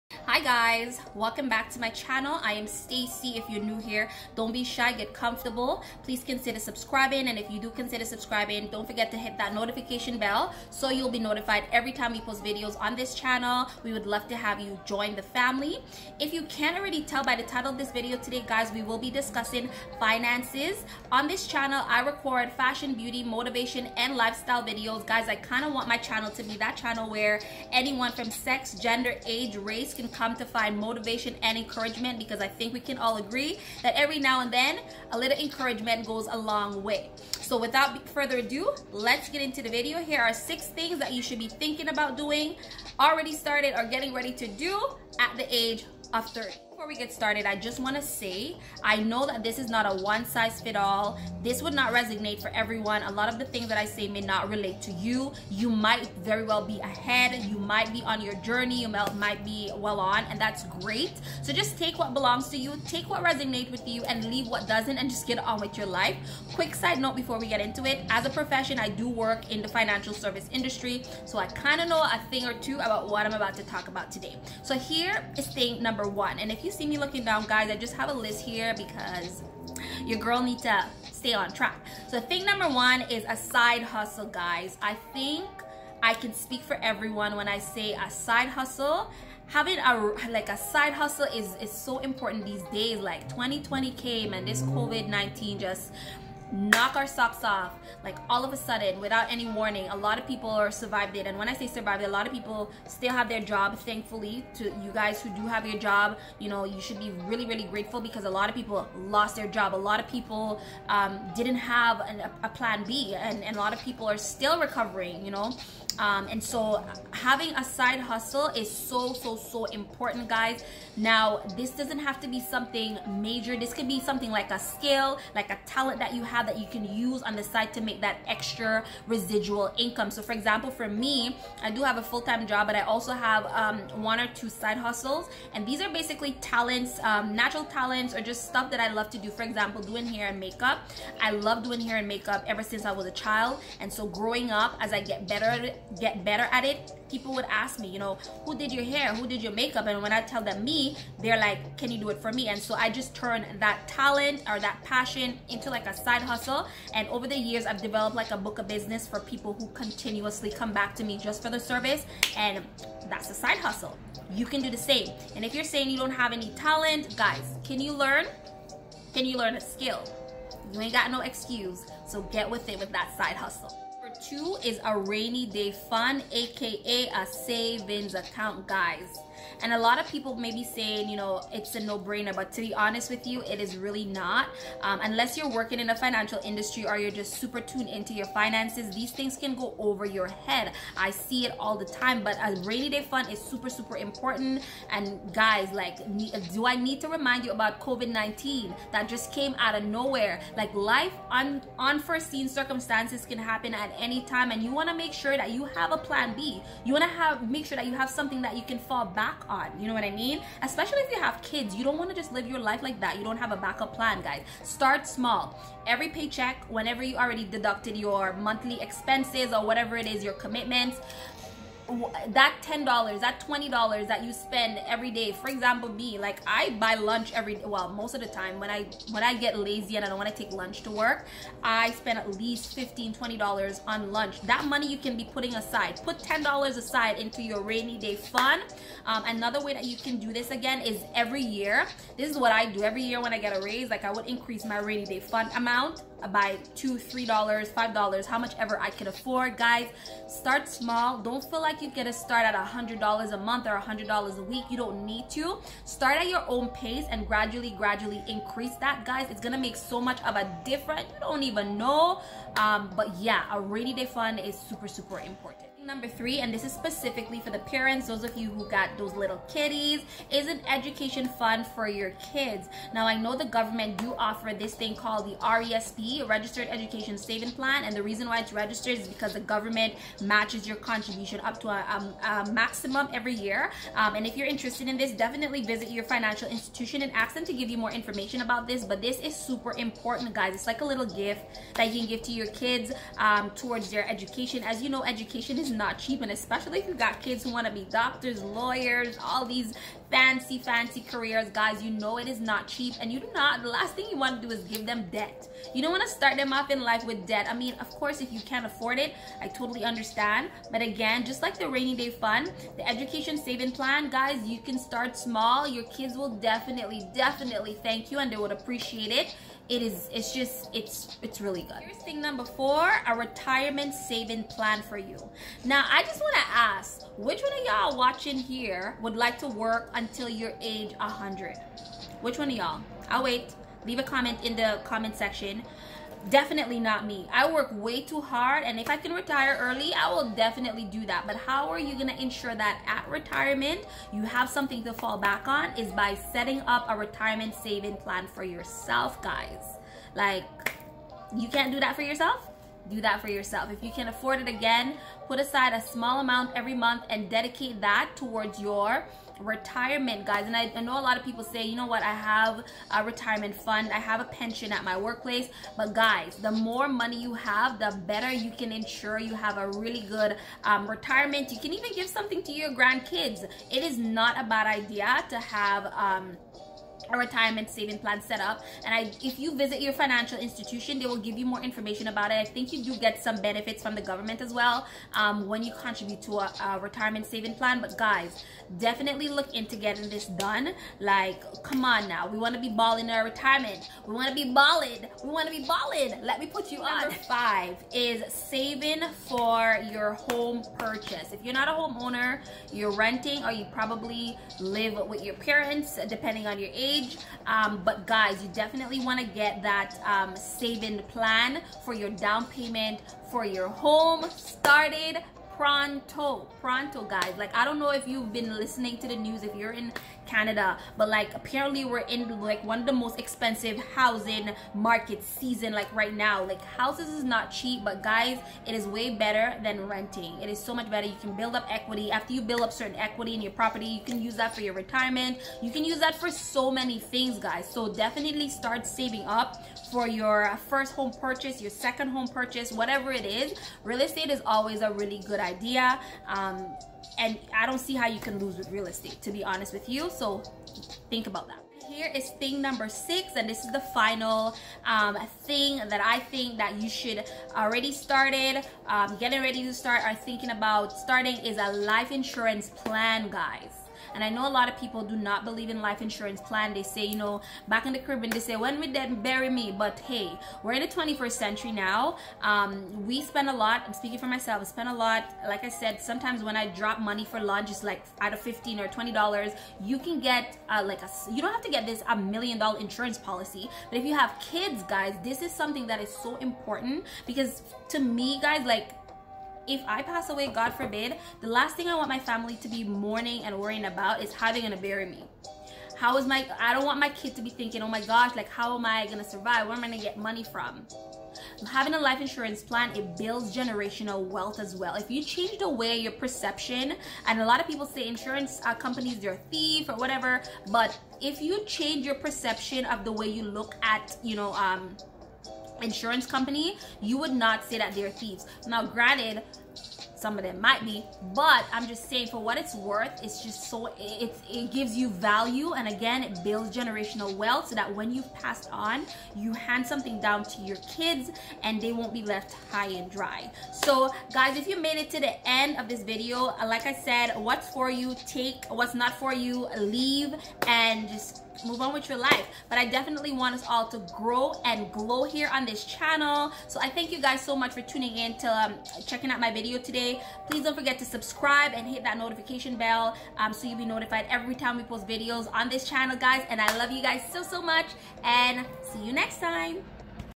The REAL Financial Glow Up| 6 Things you should do by age 30.
Today we will be discussing " How to really glow up Financially" We will look at 6 things you should already have or getting ready to start at the very young age of 30. As we all know, nothings happens until we make things happen.

The very important 6 things:
1. Have a side hustle
2. Rainy day fund
3. Education savings for you child/kids education
4. Home owners savings plan "down payment savings"
5. Retirement savings plan
6. Life insurance policy